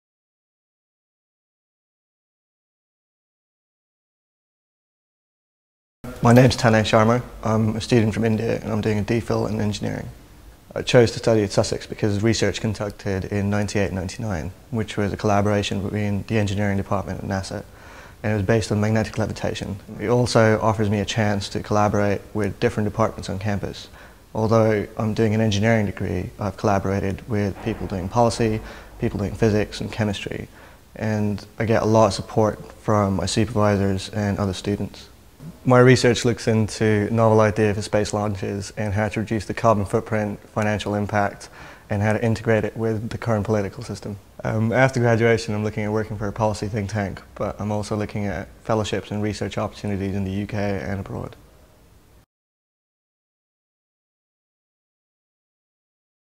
University of Sussex, PhD in Engineering
University of Sussex postgraduate research student Tanay Sharma talks about his PhD in Engineering